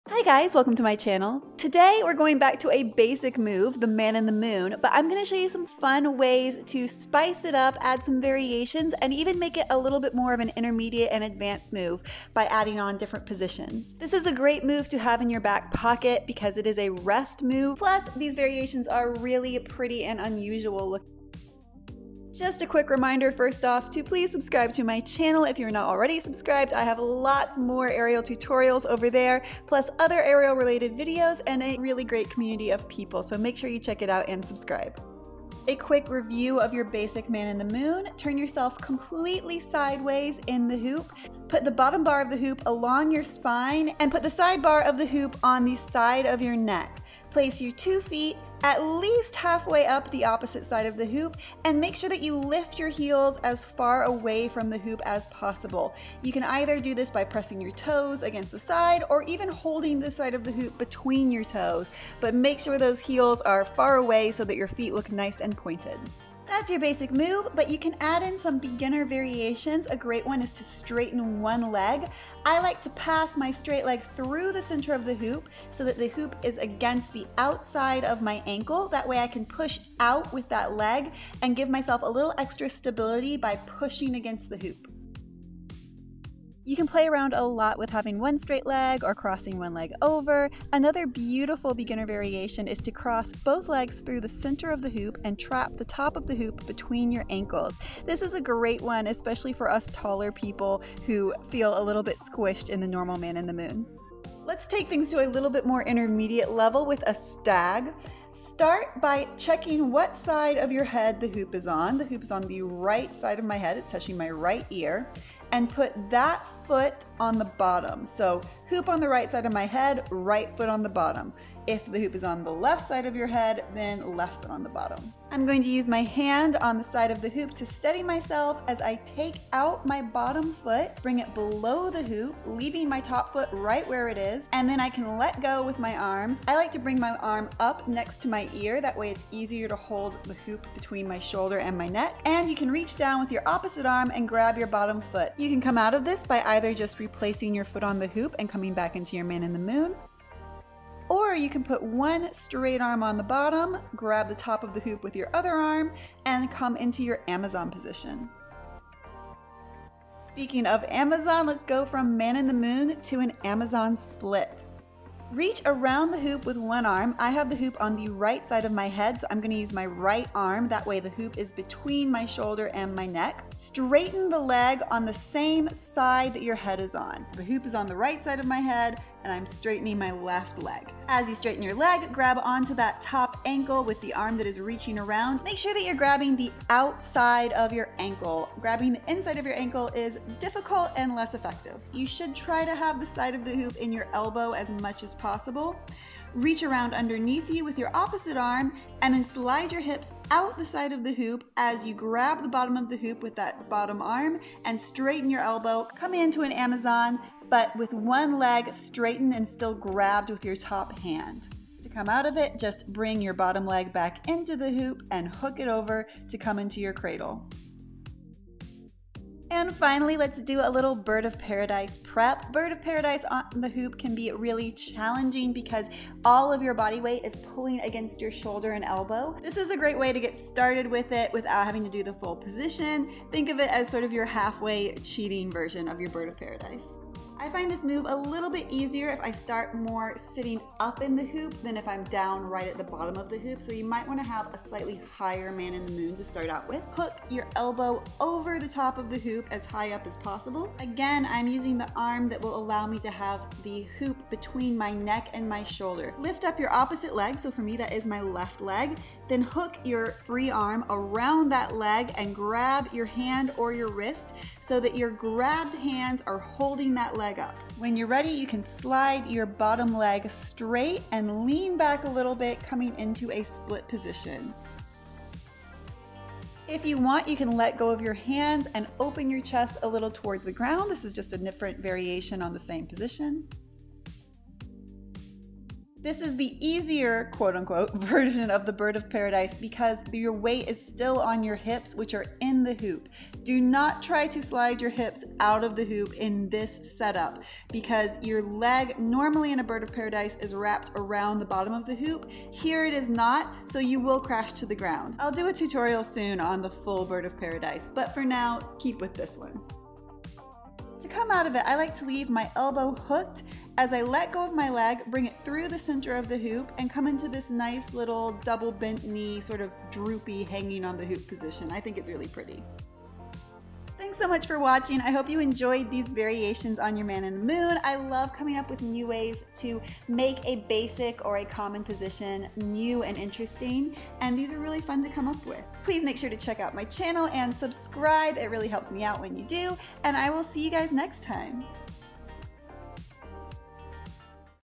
5 MOVES from Man in the Moon (Aerial Hoop TUTORIAL) Beginner - Intermediate
Here are some aerial hoop tricks to spice up a classic lyra move - the Man in the Moon. There's a variation here for any level from basic to aerial hoop beginner to intermediate aerial hoop. There are also some fun aerial splits from the man in the moon that you can add to your aerial hoop choreography or lyra routine. I'll show you haw to play around with this gorgeous aerial hoop pose and find some new ideas incorporate into your aerial training :)